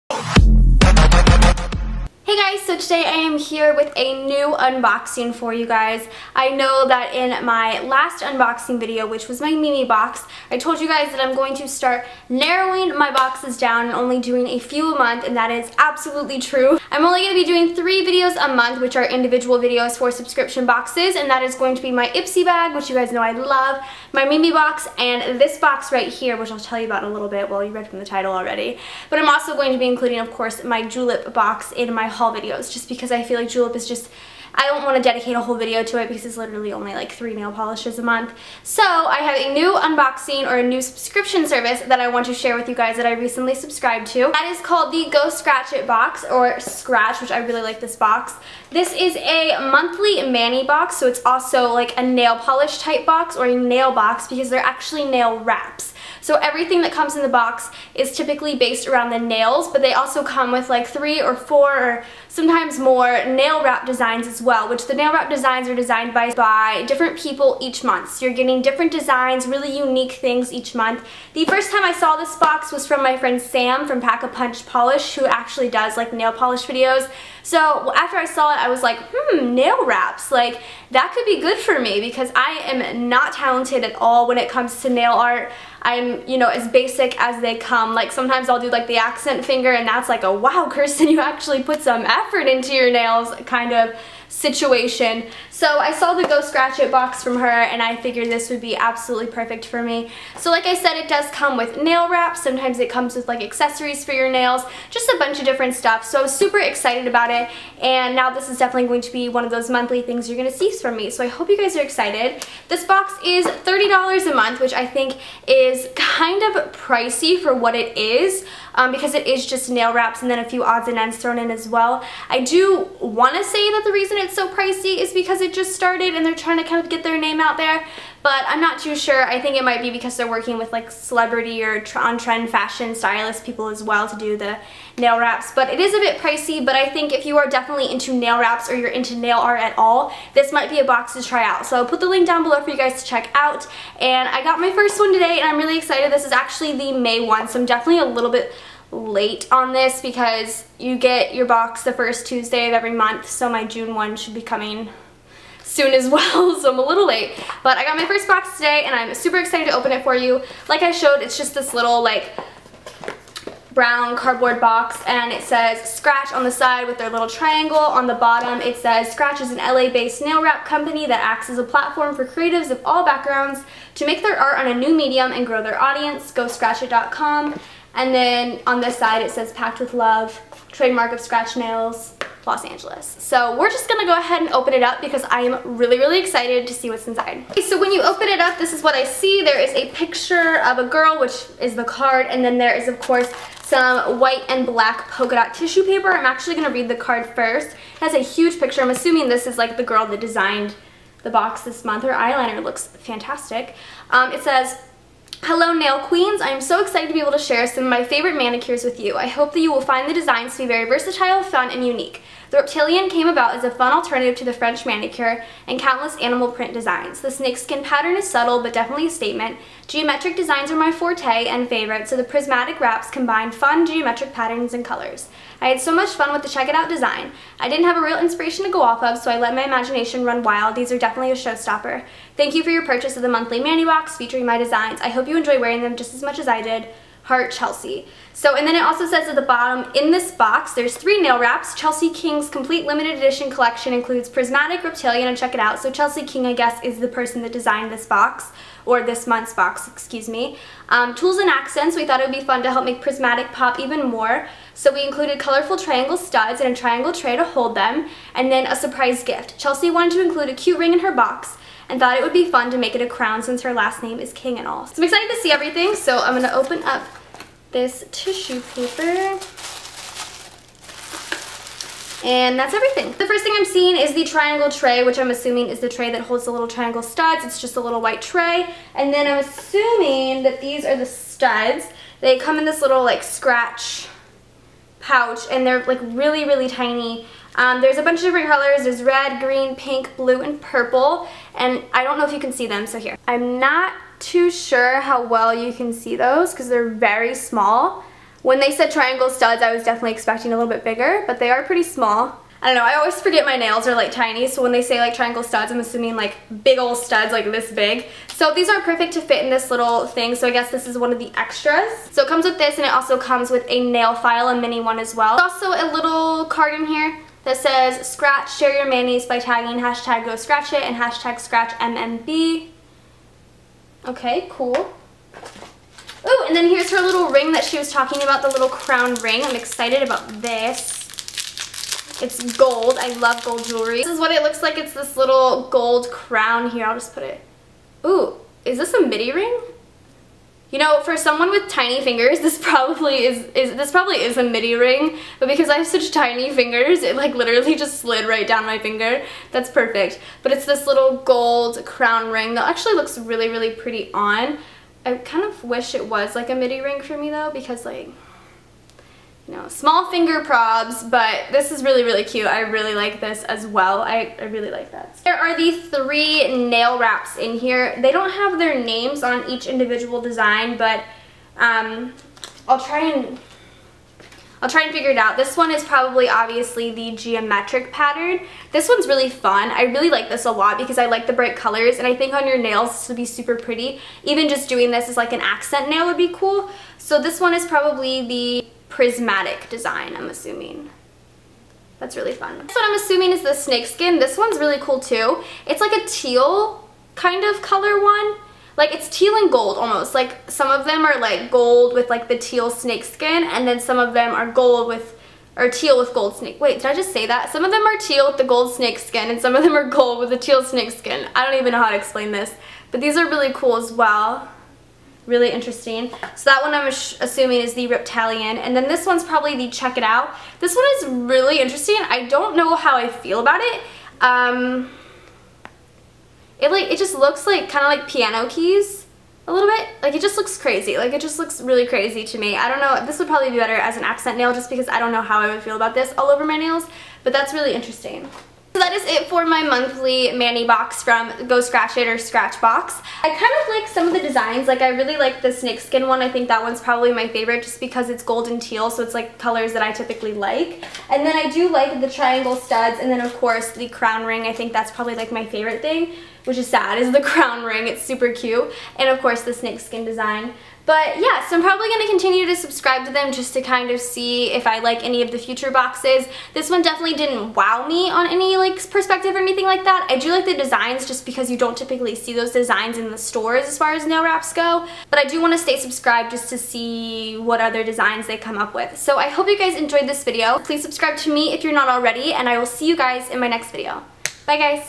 Scratch ∆ Monthly Mani Box Unboxing | May 2014
Expand the box for more info...I promise there's a lot in here!

May Box Included:
3 Nail Wrap Designs by Chelsea King
- Reptilian
- Prismatic
- Check It Out
Crown Ring
Triangle Studs & Tray
Nail Accessories

On my nails: Nothing
My shirt is from:  PacSun
My necklace is from: H&M
On my lips: MAC Lipstick in Rebel

FTC: Scratch did send me this box for consideration however all opinions are my own.